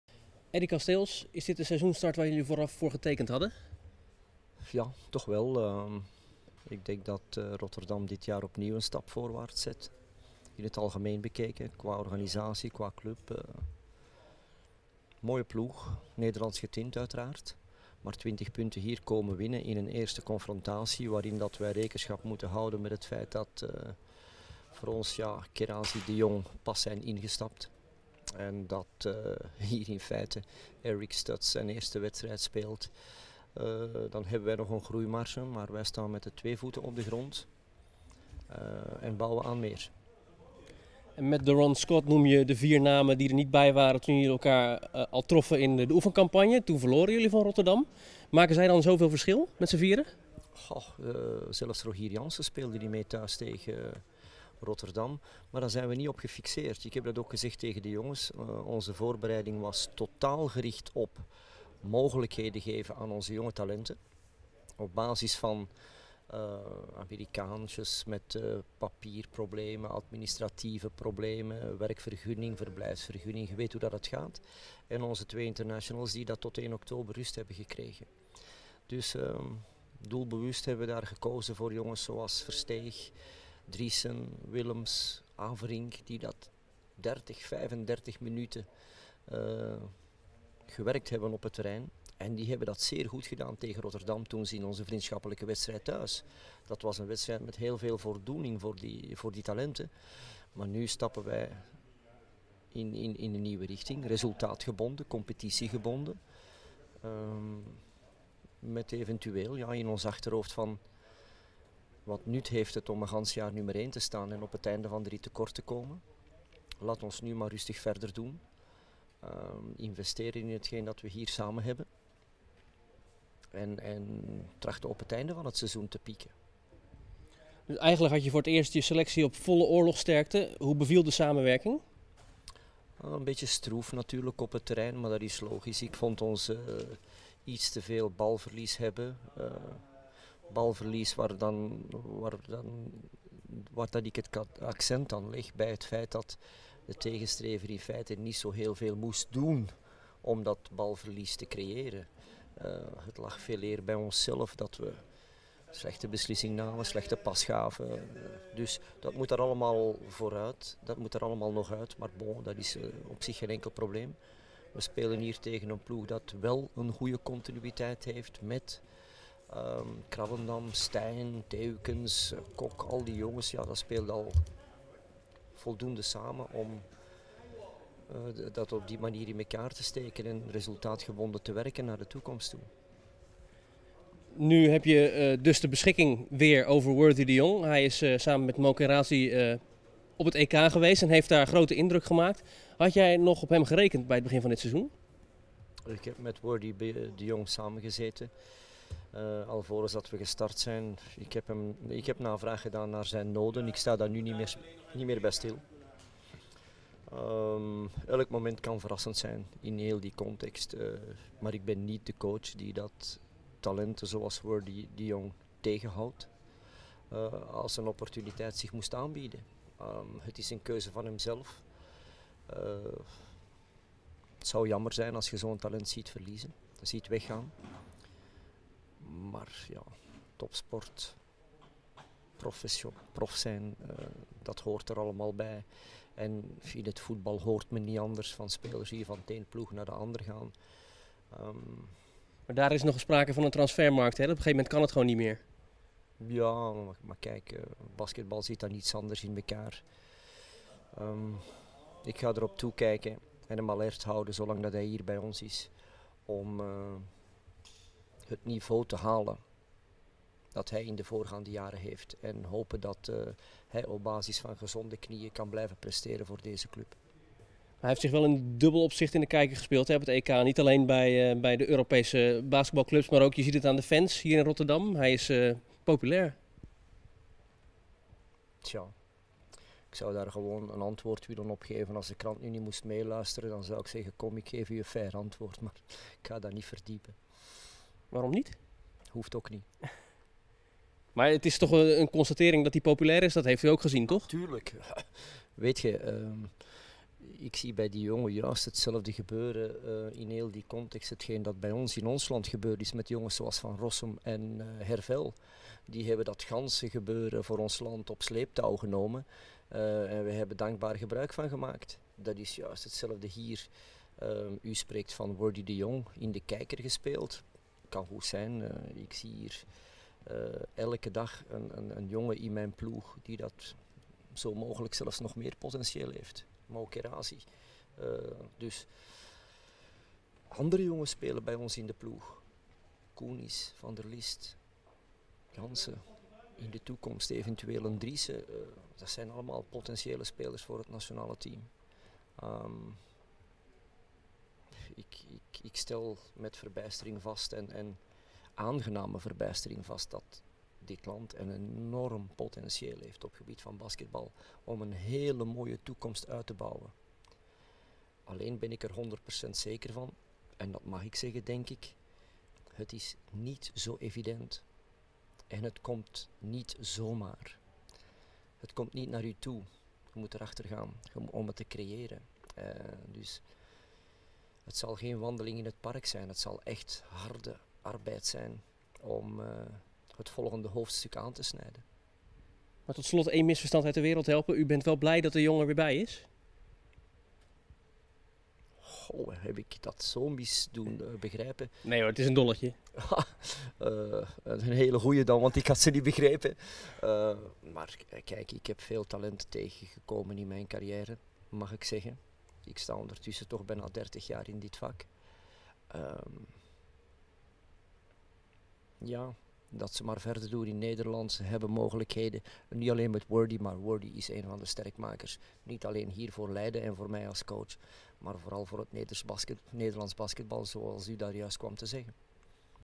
Eddy Casteels (ZZ Leiden): 'Talent als Worthy de Jong niet tegenhouden'
Interview met basketbalcoach Eddy Casteels na de 60-79 overwinning van Zorg en Zekerheid Leiden op Rotterdam in het Topsportcentrum